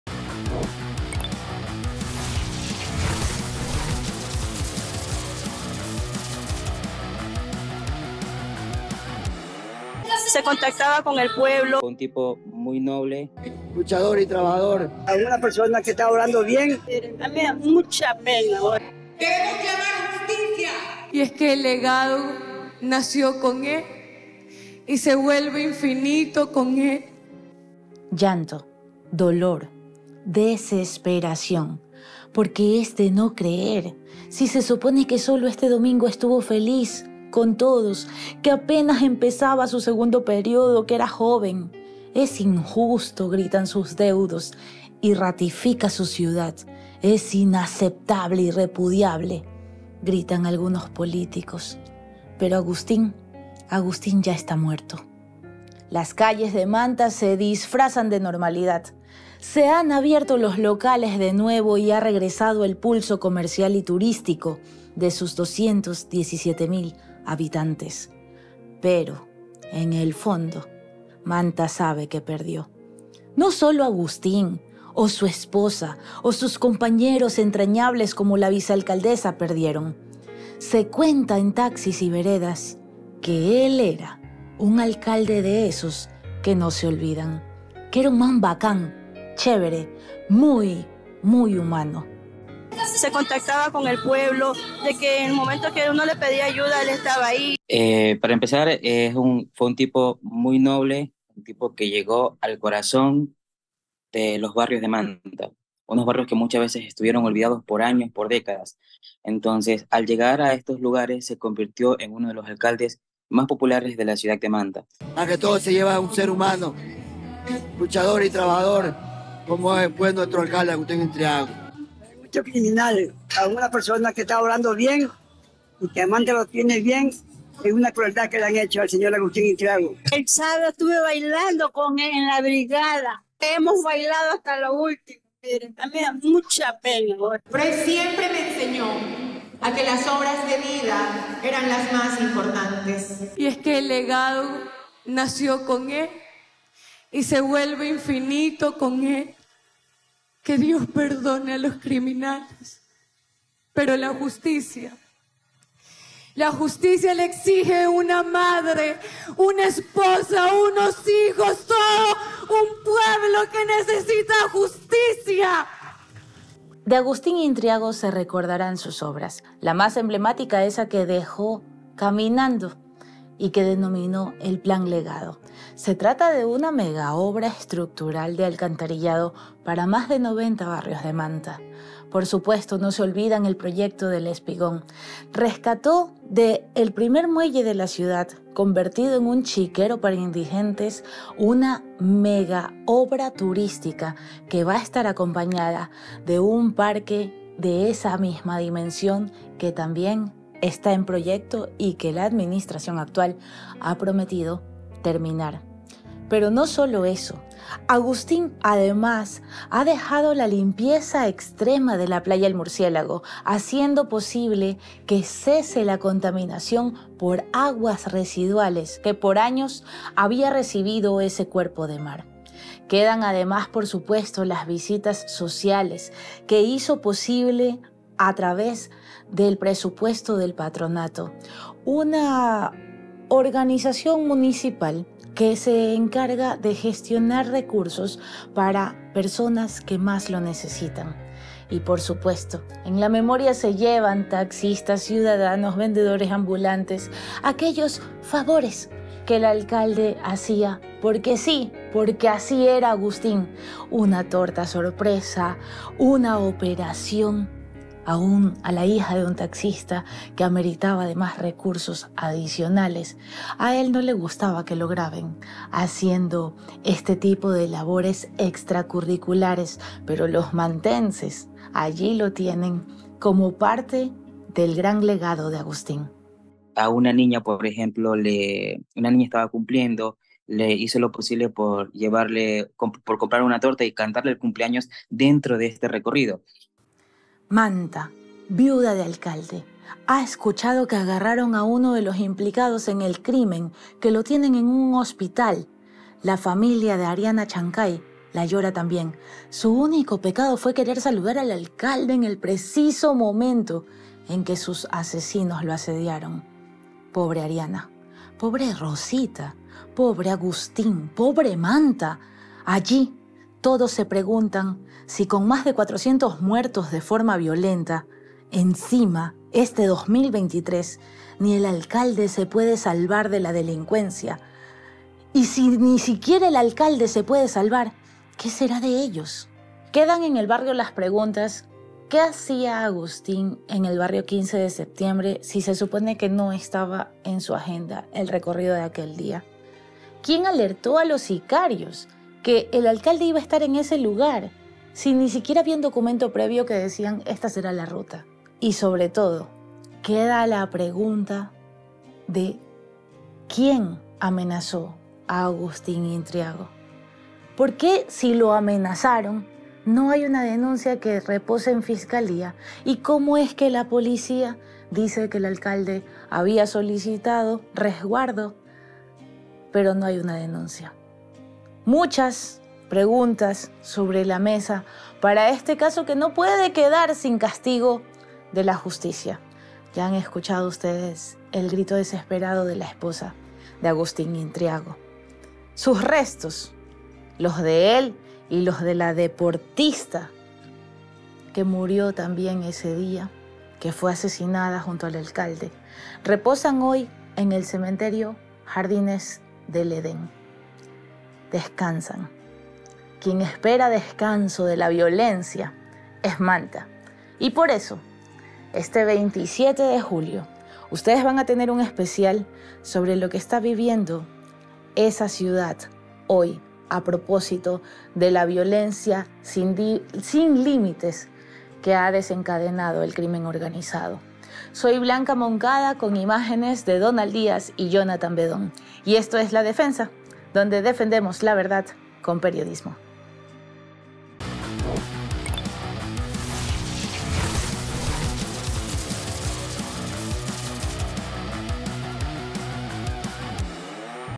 |LA NOTA| - Agustín Intriago, el legado de un alcalde asesinado
¿Quién fue y qué significó para Manta Agustín?

Su gente te lo cuenta en este especial de La Defensa, en donde recogemos su obra y algunas anécdotas. Además de las reflexiones sobre las respuestas que nos deben de su crimen.

Defendemos la verdad con periodismo. 🛡️

Reporta: Blanca Moncada y Donald Díaz
Cámaras: Donald Díaz y Johnattan Bedon
Edición: Galo Avecilla
Audio: Mario Avila